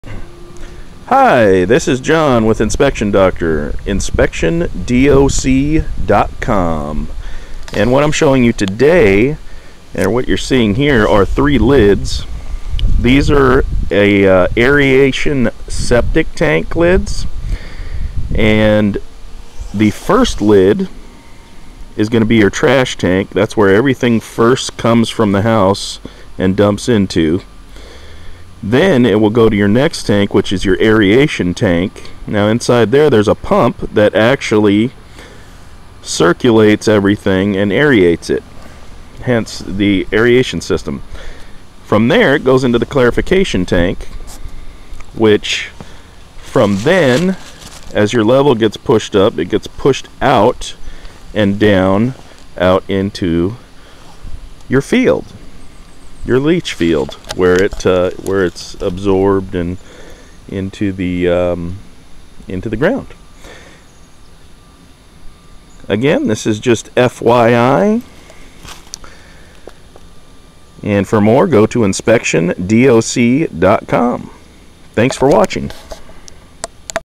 Aeration onsite waste treatment system, Septic tank
Onsite waste system, aeration unit, overview and FYI basic, septic home tank, to learn more about septic and other systems